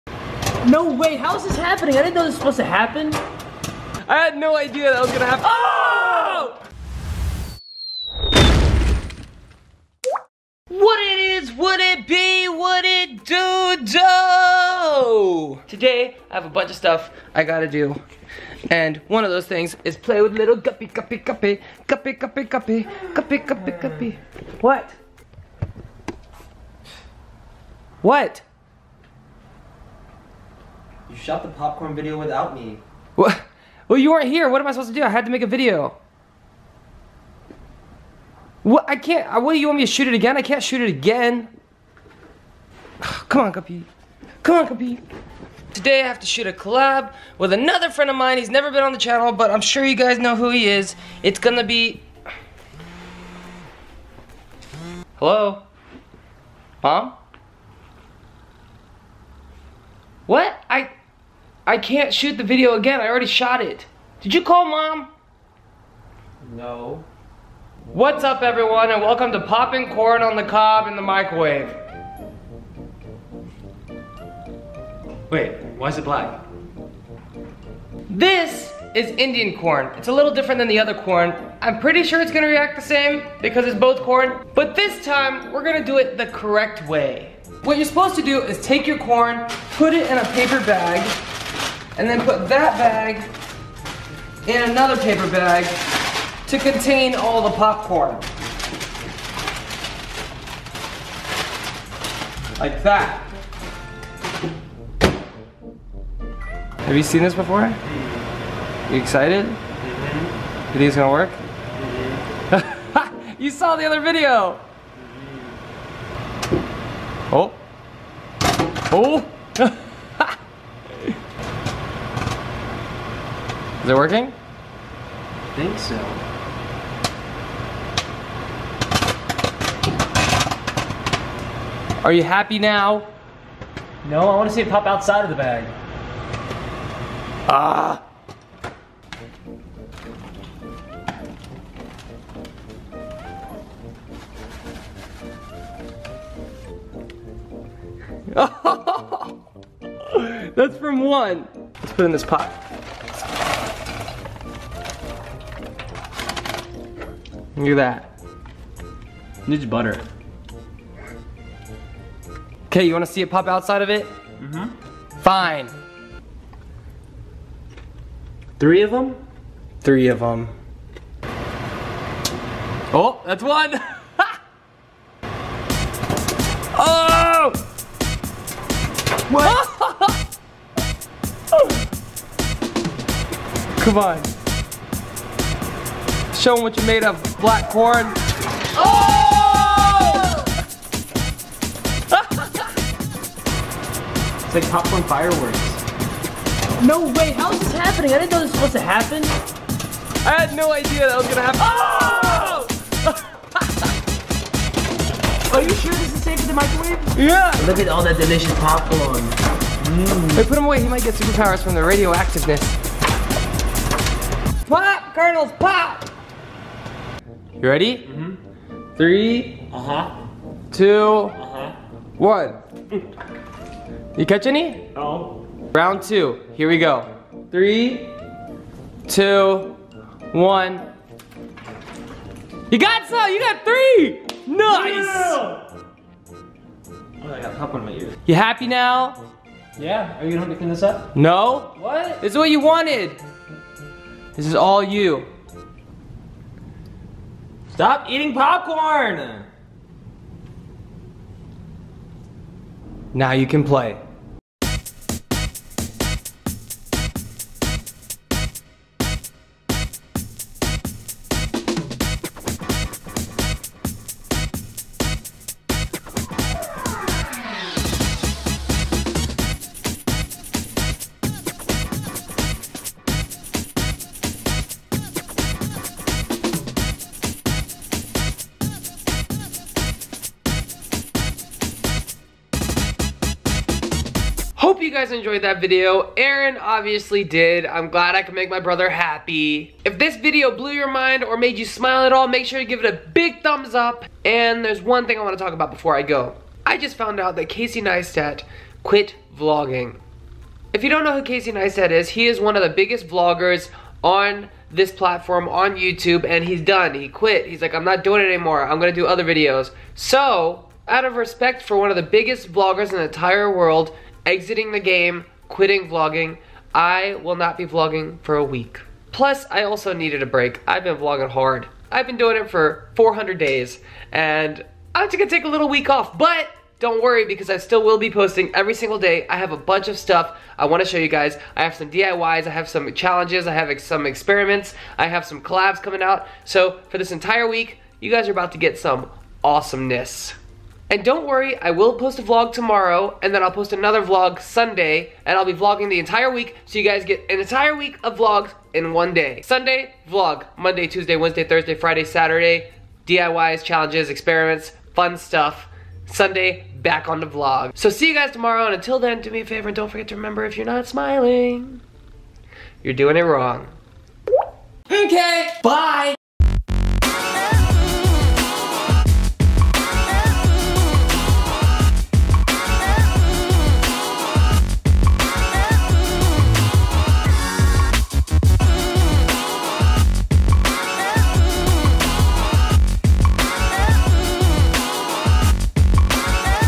POPPING BLACK CORN ON THE COB!!
I will NEVER get bored of Popping Corn On The Cob! LOL

mKay bYe!